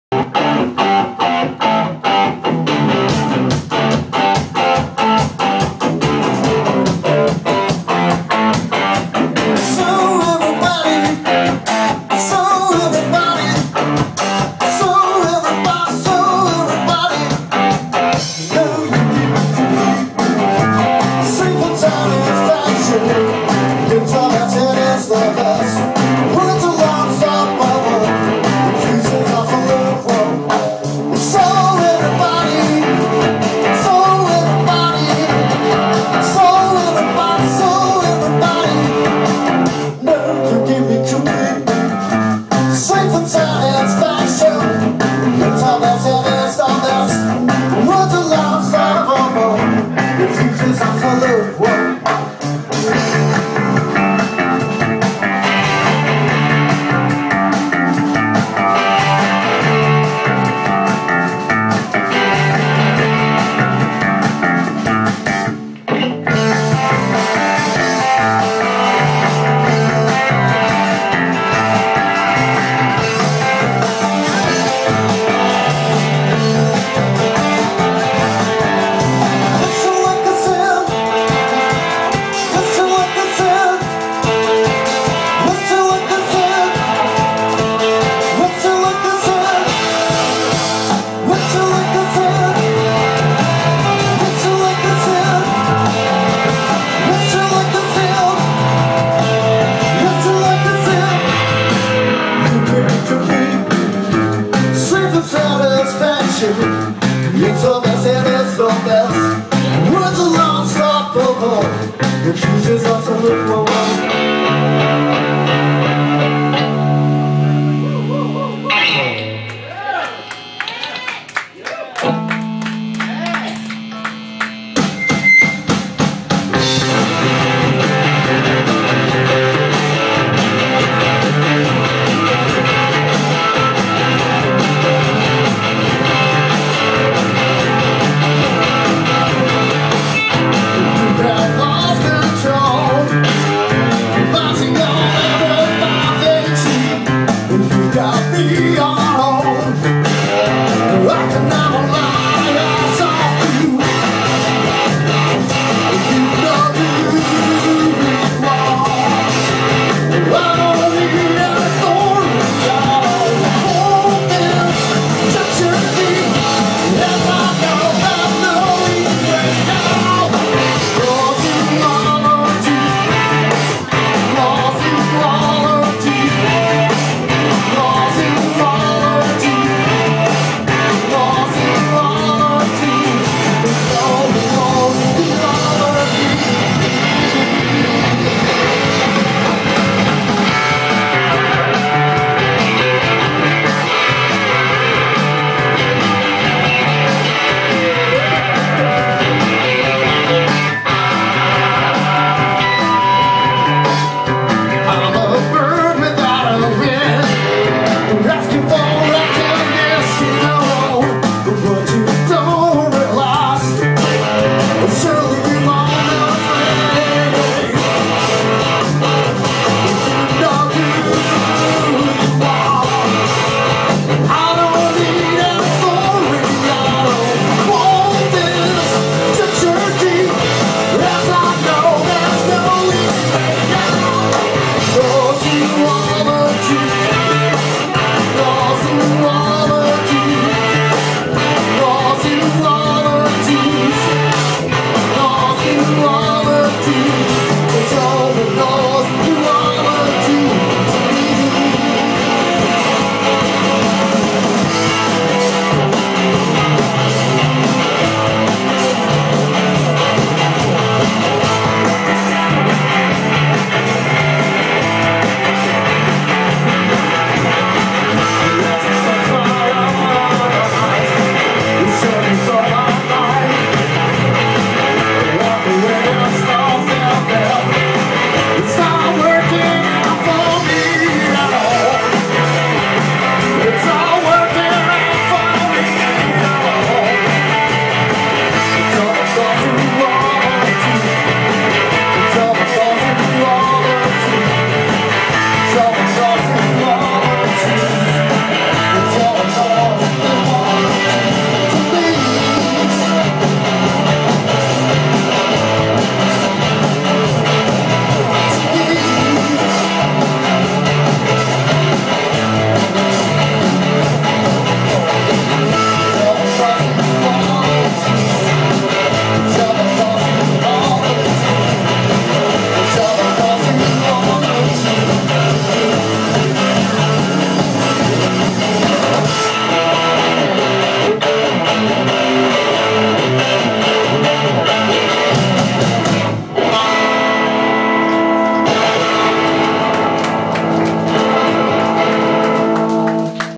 BLPRS 2011年3月4日
2011年3月4日 難波Meleで行われた「HIGH NUMBER」でのライブ。